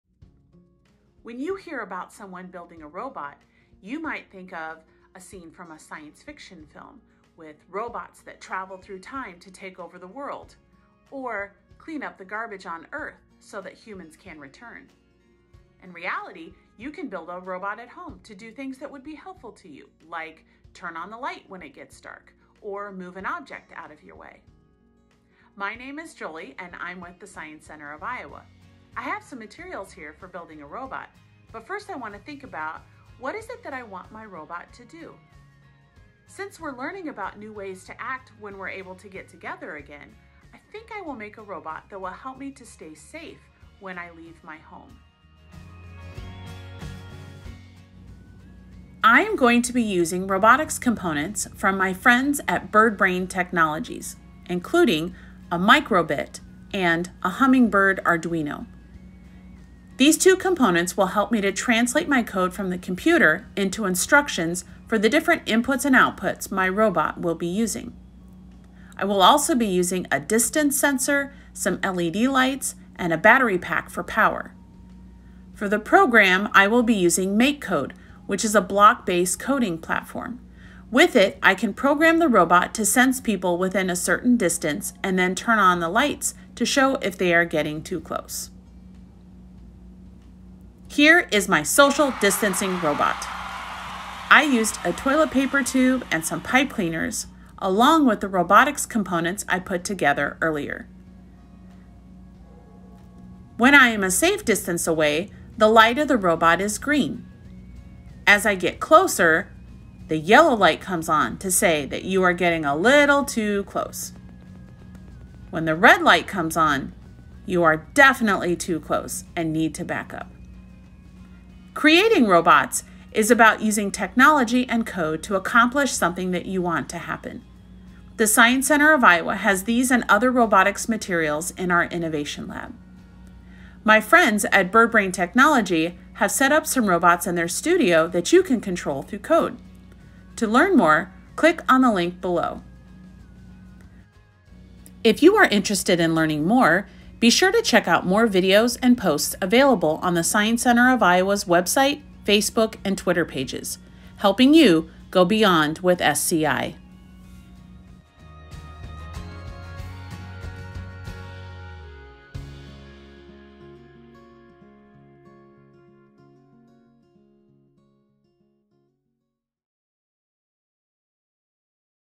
Go Beyond with SCI: Robots
Our director of innovative STEM teaching, Jolie, created a socially distancing robot!

Join her to learn how robots can be useful to our lives, see how she made her robot and discover how you can control some pre-made robots from home with Birdbrain Technologies